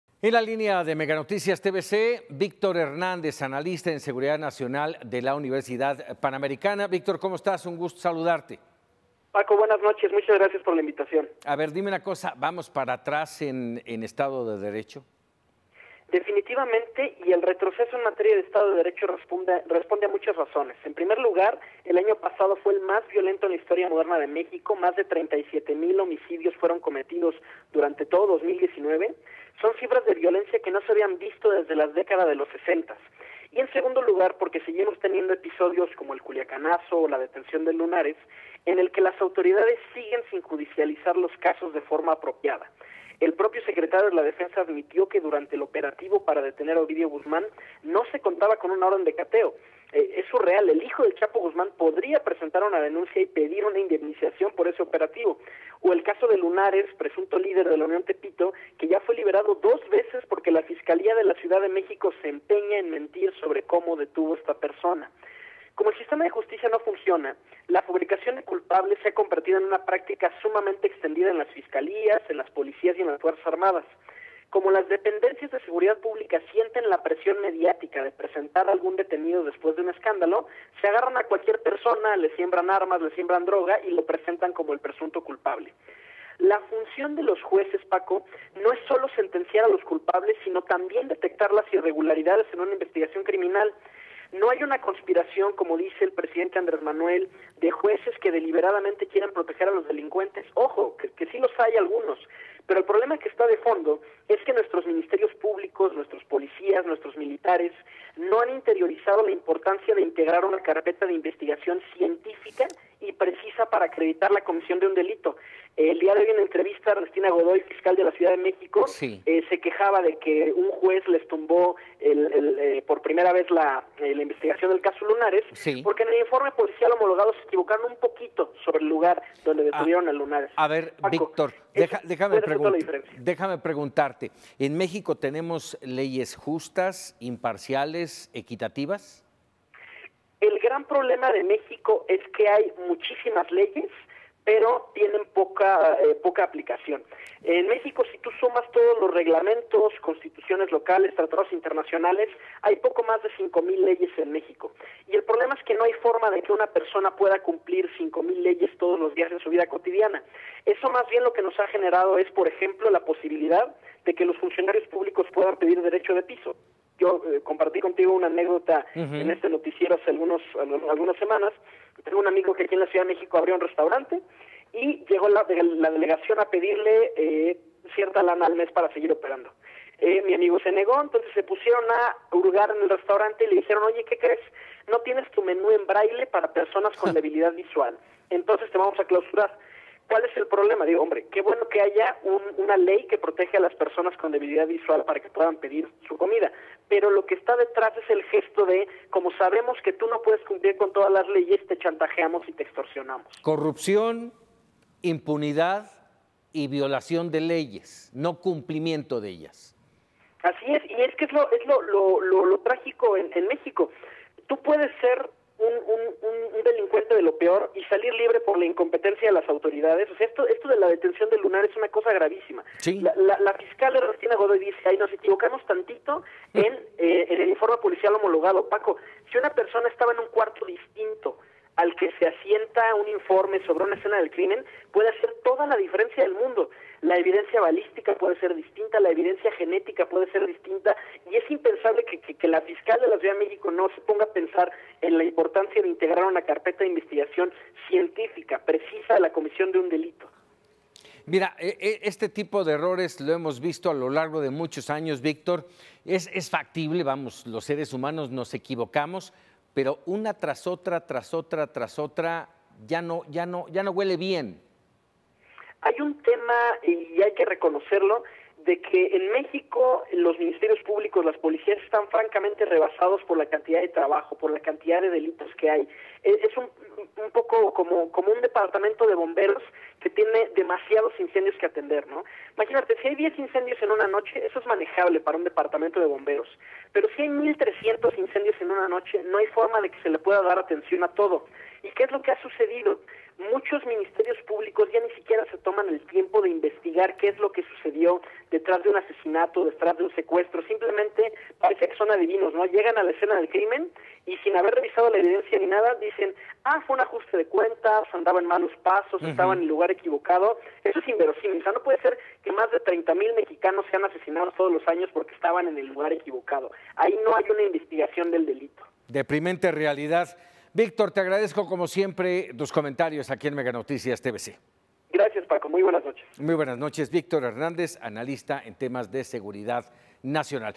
Un sistema de justicia débil
La inseguridad continua en el país y uno de los problemas es la mal aplicación de la justicia en los casos. Entrevistamos a Víctor Hernández para entender mejor el tema.